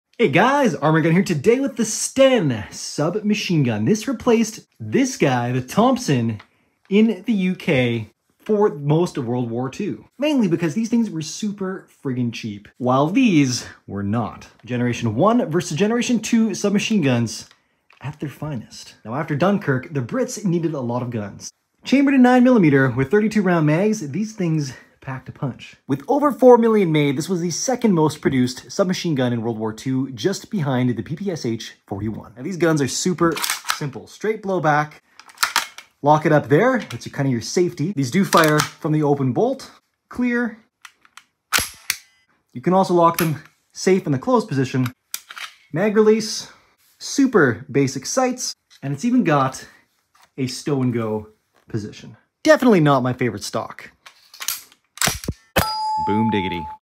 The STEN 💥 WW2’s 2nd Most Popular SMG in 1 Minute
STEN = Shepherd & Turpin (Main Creators) + Enfield (Main Producer)
Get your gas masks here! Mira Safety
My CODE is always ARMANDGUN